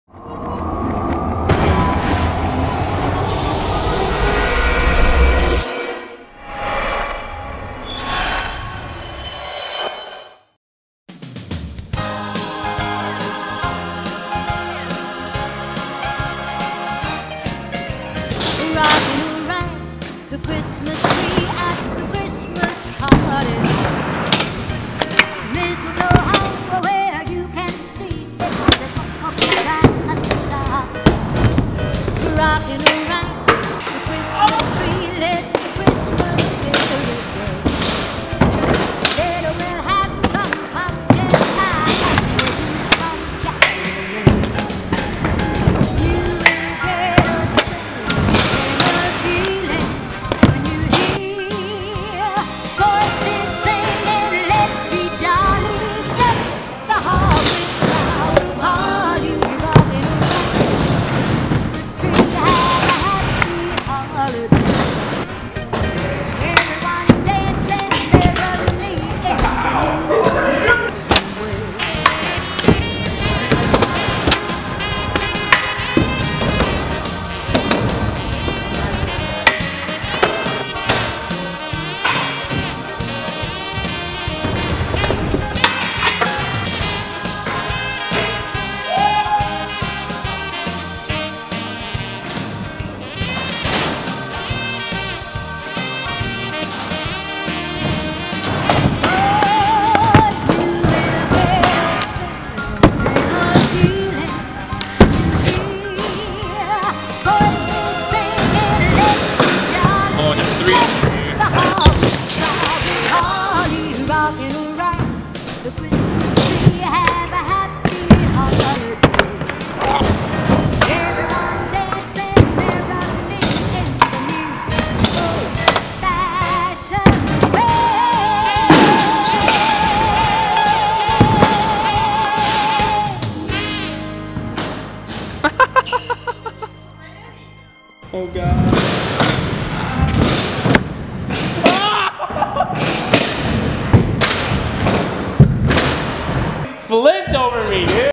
Merry Christmas 2013
Christmas edit that I put together. I had a blast filming with all the homies! Hope you enjoy - Ryan Gould